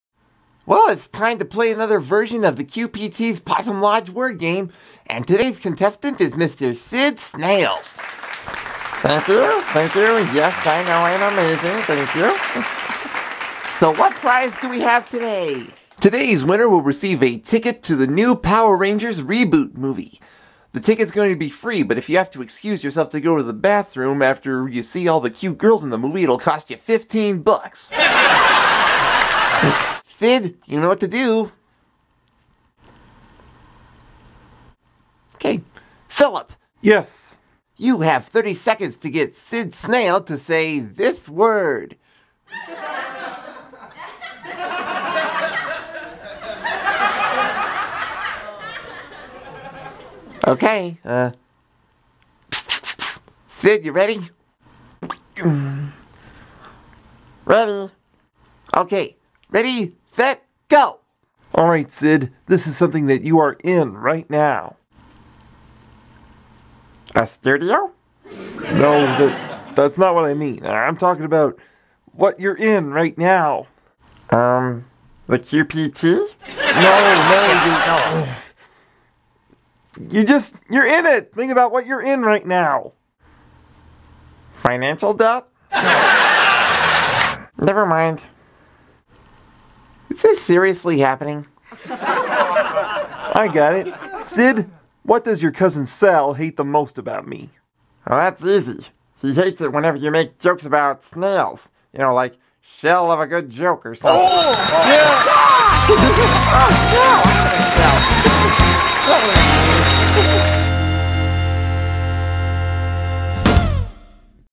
The Possum Lodge Word Game - Quesal Puppet Style - SHELL
Fans of The Red Green Show may enjoy this...
I'm a big fan of The Red Green Show so I made The Quesal Puppet Troupe's Sid Snail and Phillip Bea Frog play a QPT version The Possum Lodge Word Game. Enjoy!